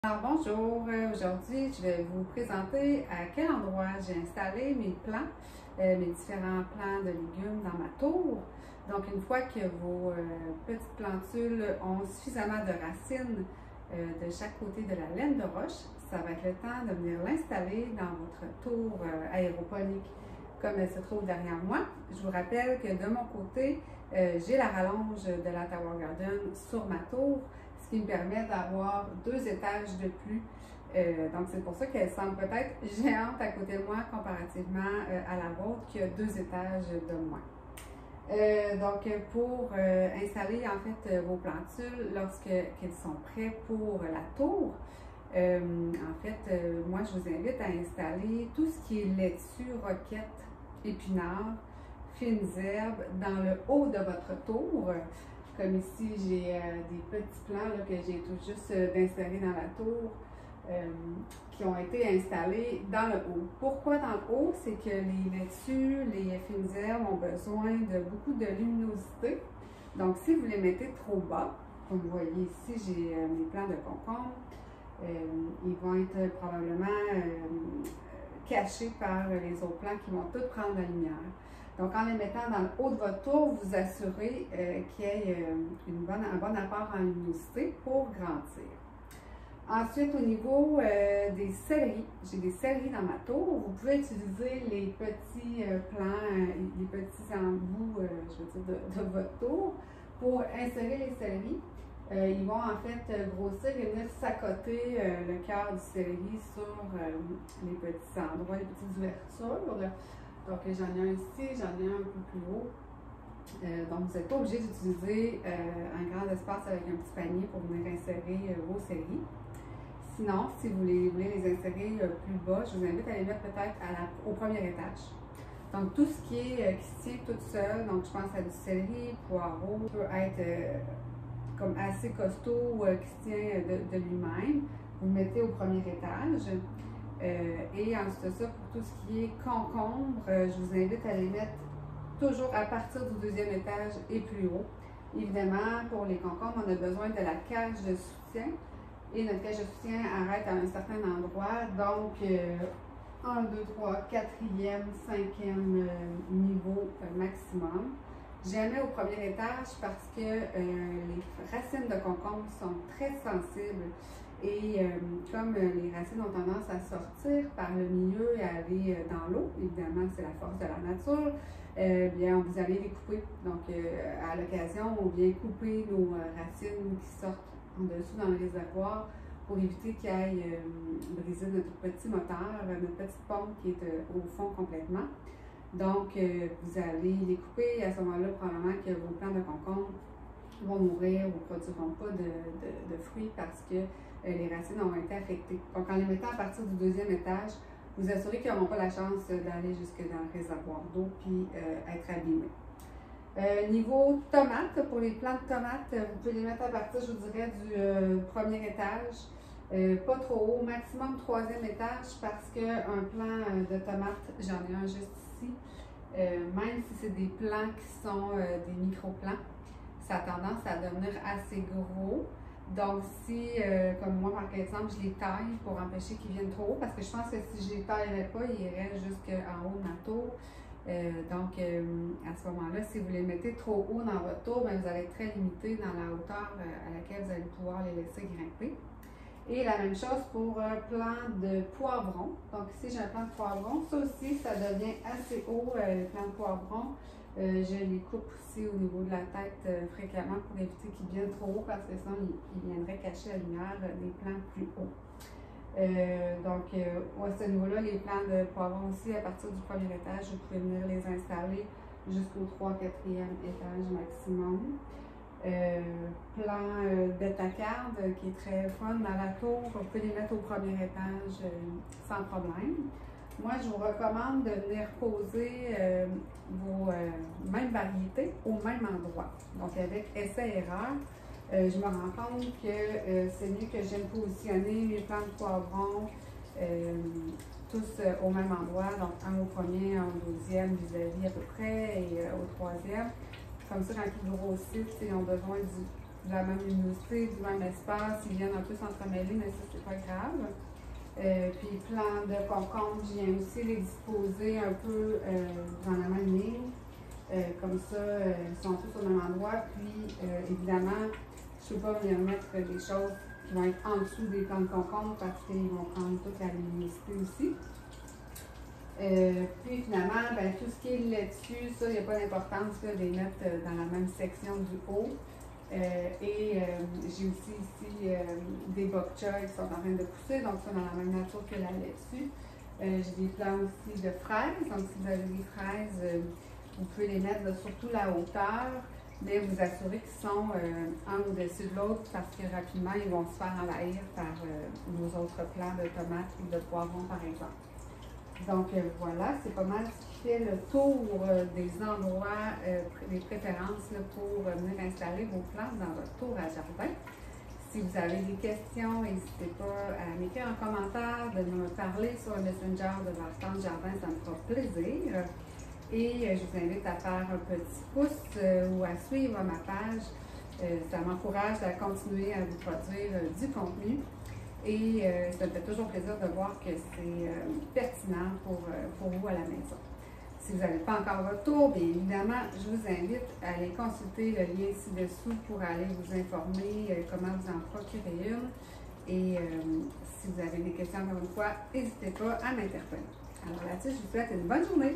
Où mettre vos différents plants dans la Tower Garden
Vous vous demandez à quel endroit mettre vos plants dans votre tour? Voici mes recommandations pour optimiser votre Tower Garden.
Des questions ou commentaires, ils sont les bienvenus dans la section commentaires ou encore sur le Messenger de la page Facebook de Vert tendre Jardin.

Vous hésitez? Voici les informations sur les Tower Garden